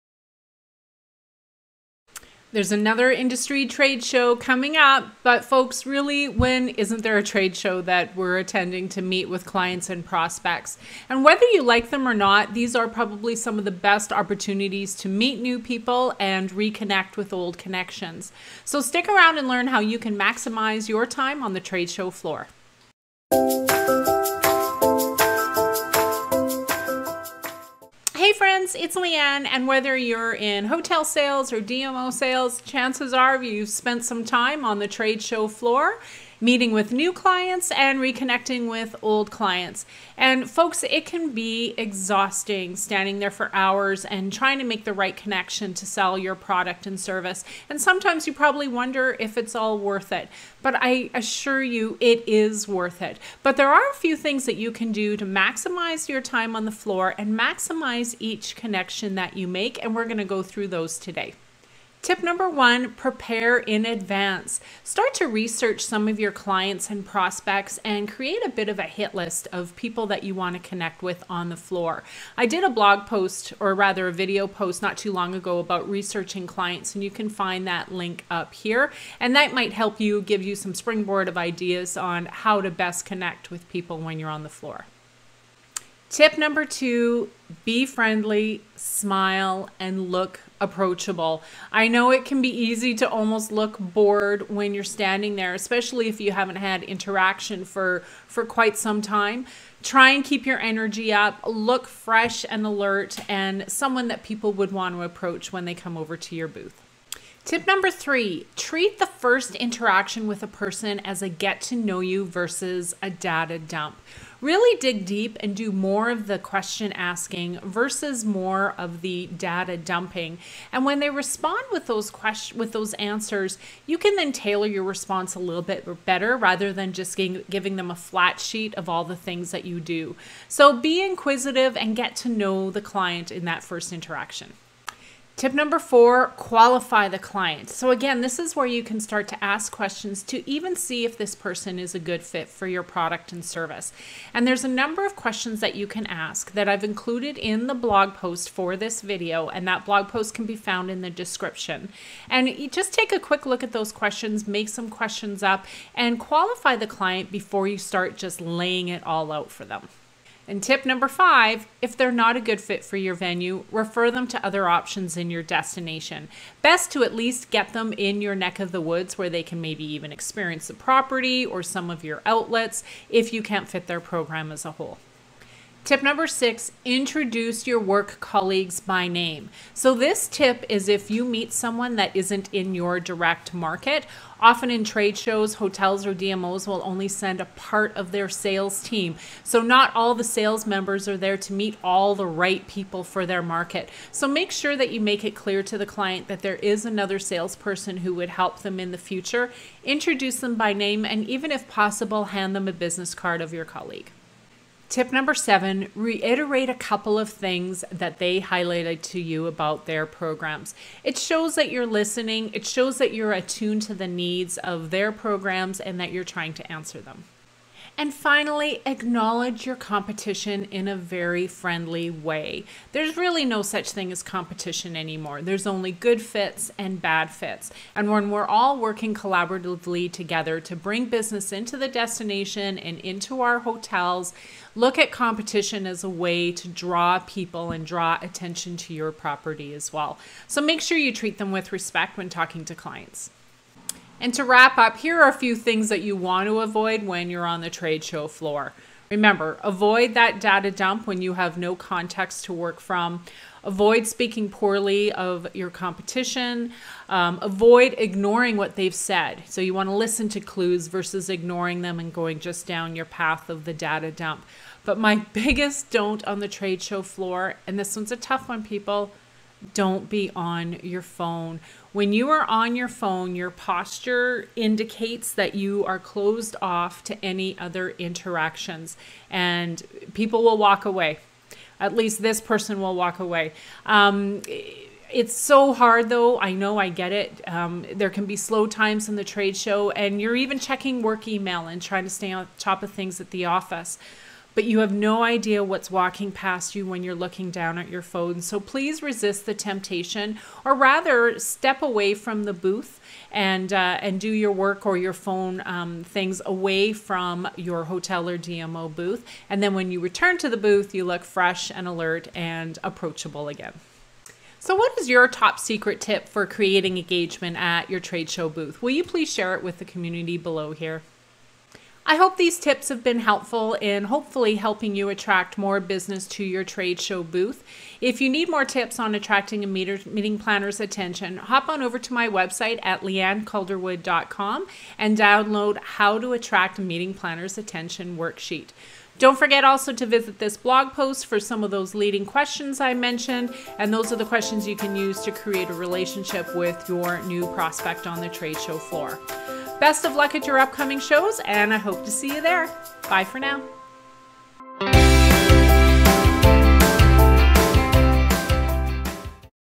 Hotel Sales Training - Tradeshow Sales Tips for the Hospitality Industry
If you’re in hotel sales, you’ve manned the tradeshow booth.  Industry shows are part and parcel of your role, and whether you like them or not, they’re one of the best ways to meet new clients and prospects!

Walking into a tradeshow ill-prepared can be disastrous, for both you AND your prospect!  Ensure you are at your best with these trade show sales tips to help you prepare for YOUR next trade show!